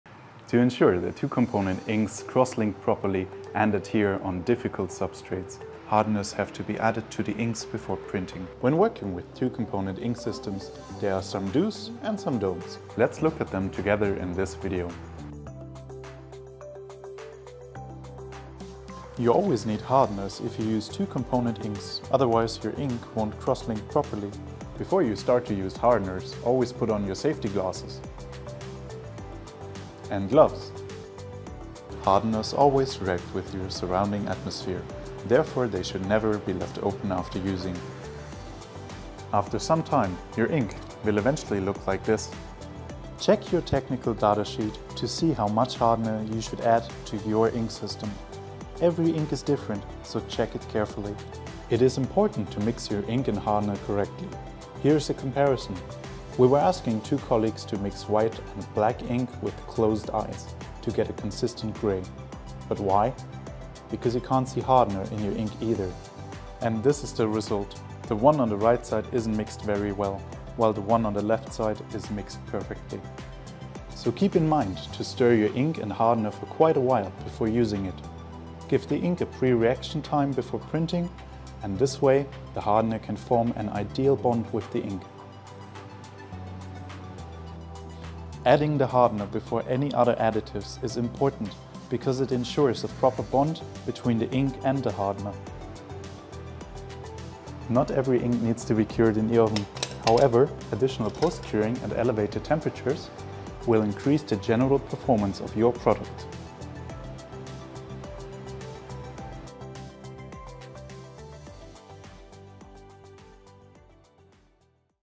How to use hardeners when printing with two-component inks
To ensure that two-component inks crosslink properly and adhere on difficult substrates, hardeners have to be added to the inks before printing. When working with two-component ink systems, there are some do’s and some dont’s. Let’s look at them together in this video.